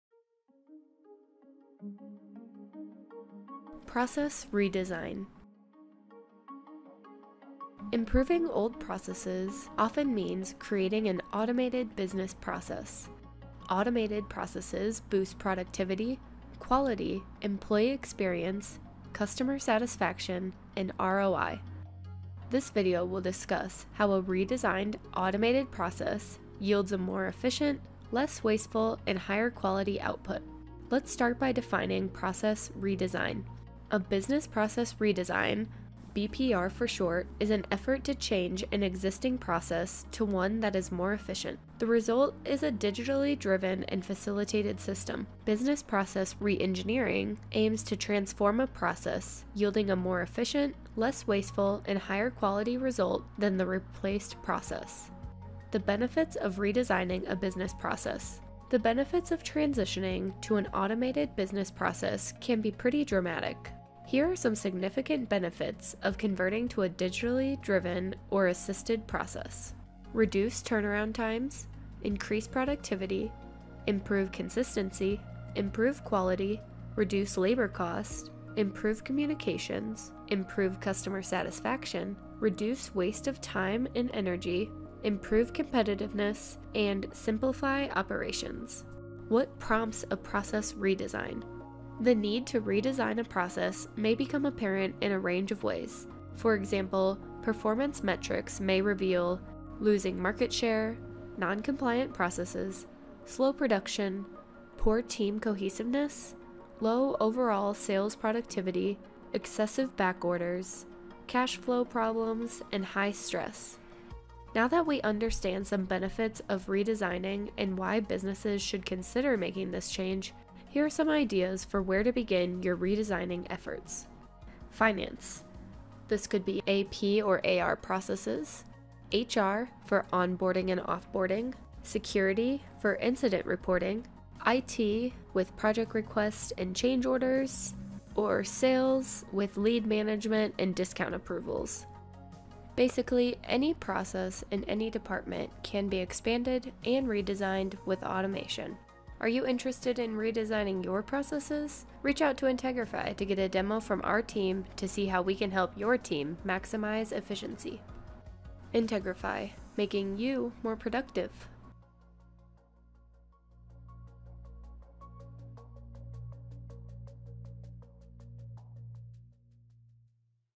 What is a Process Redesign?
Process Redesign: Redesigning an outdated process or operating procedure creates an Automated Business Process (ABP). This ABP can increase productivity, quality, employee experience, customer satisfaction, and Return On Investment (ROI).

This video covers the definition of process redesign, the benefits, what will prompt a process redesign and ideas on where to begin your journey.

00:00 Introduction
00:32 Process Redesign Definition
00:55 The Benefits of Process Redesign
01:30 What Prompts a Process Redesign?
02:01 Automation Ideas